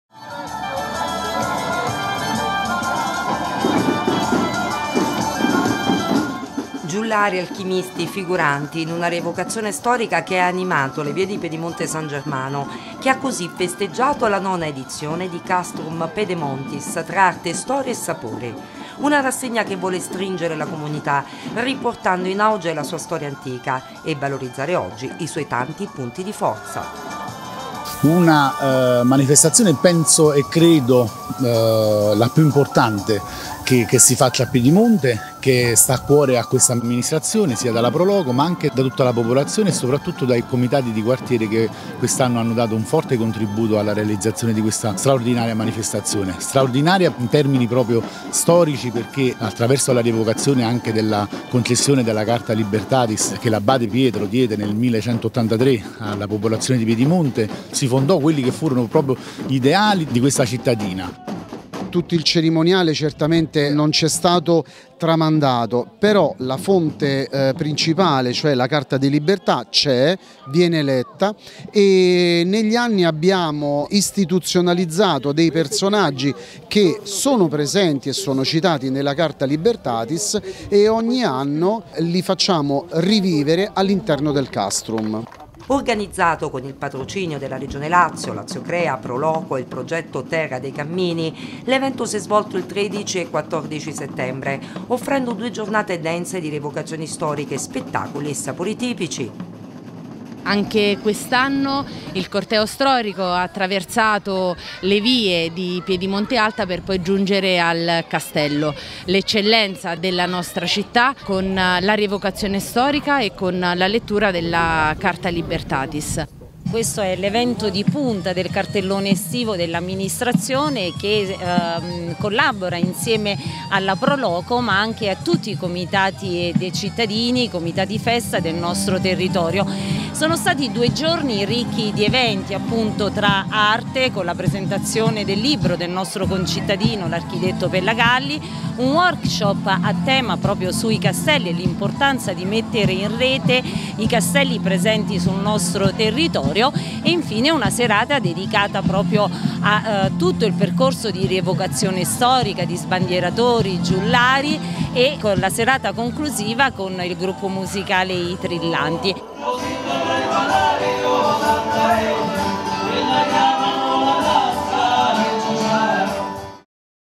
PIEDIMONTE S.G.: Castrum Pedemontis del 15-09-2024
Piedimonte san germano si ritrova, come ogni anno, per raccontare la sua storia antica. La rassegna  Castrum Pedemontis  mette in bella mostra arte, storia e sapori  di un territorio ricco di eccellenze.